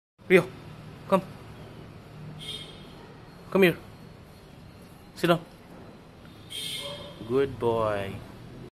Lhasa Apso Puppy training
Here is my sweet puppy called RIO, one of his video during training sessions.